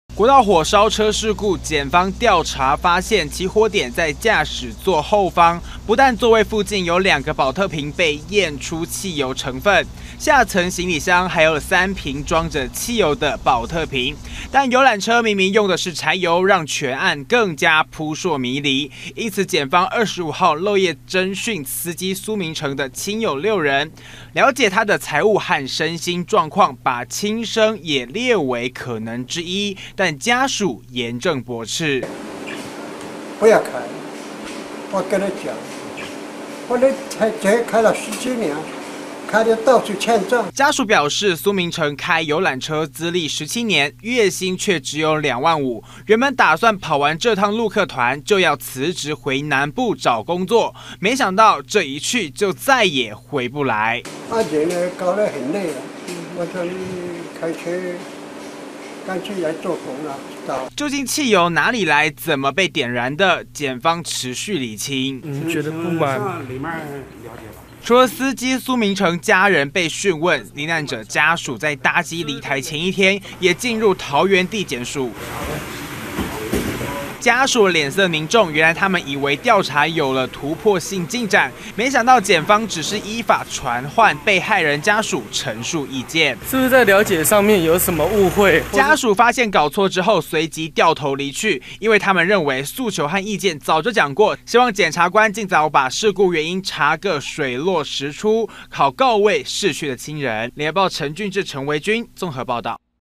【2016.07.26】火燒車案調查司機 父駁輕生傳聞
國道火燒車造成26人死亡，車上有五瓶汽油，讓全案再添疑雲，究竟是意外起火、人為縱火或自焚…

疑點有待釐清。檢警25號搜索司機蘇明成住處、漏夜訊問親友，不過他的父親表示，蘇明成絕對沒有輕生的念頭，原本打算跑完這趟車，就辭職回南部，沒想到卻天人永隔。完整內容 , FB , Twitter , G+